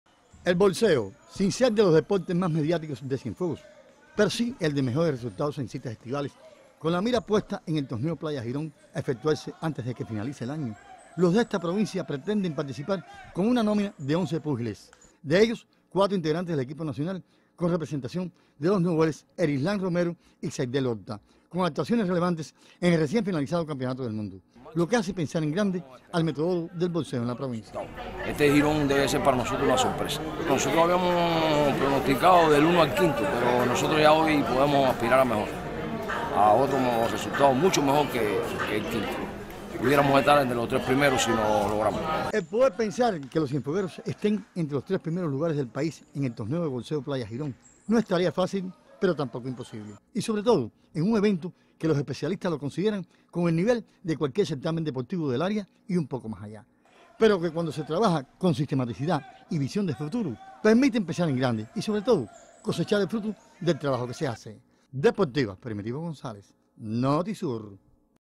En preparación atletas y entrenadores de Cienfuegos con vista al Torneo de Boxeo Playa Girón
Atletas y  entrenadores de Cienfuegos se preparan para el Torneo de Boxeo  Playa Girón, evento deportivo que ha sido pospuesto por las limitaciones económicas que enfrenta el país y el mundo.